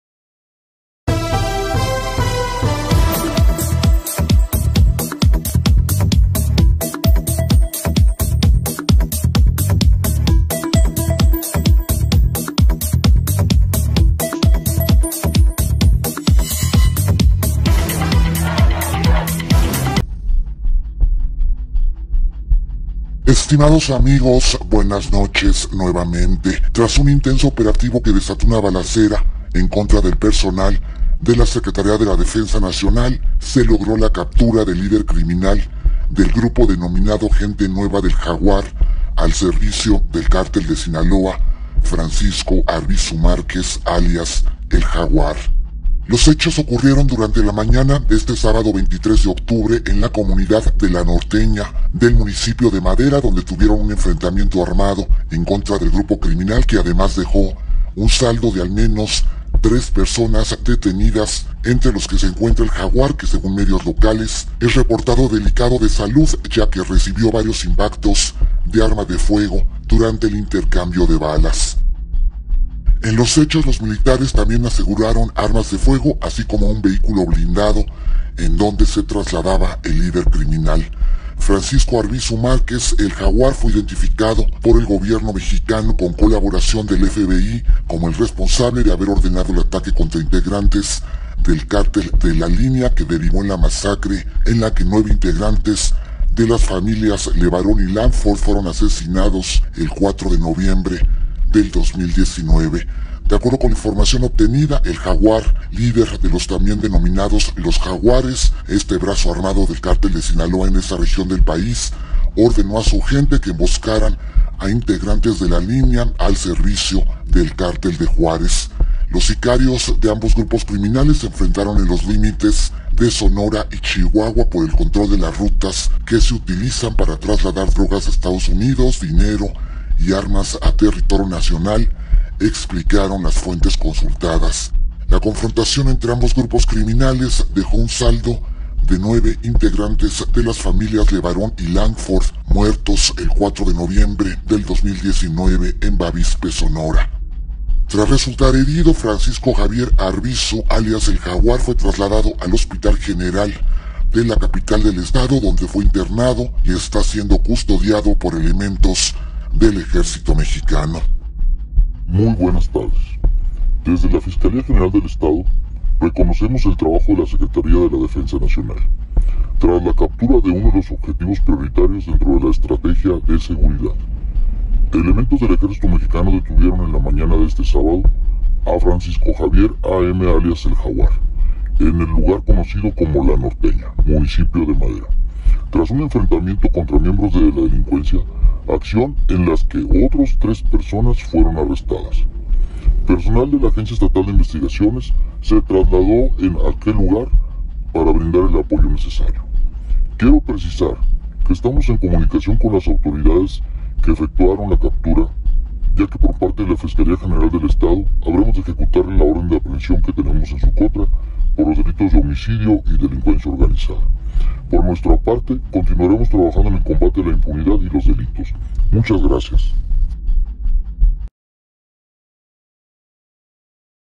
líder de Gente Nueva al servicio del CARTELDESINALOA mato a LOSLEBARON
Tras un enfrentamiento con efectivos de la SEDENA y de la GUARDIANACIONAL, fue capturado Francisco Arvizu Márquez, ELJAGUAR líder de GENTENUEVA un grupo de sicarios al servicio del CARTELDESINALOA, que fue identificado por el gobierno mexicano, con colaboración de la FBI, como el responsable de la masacre de la familia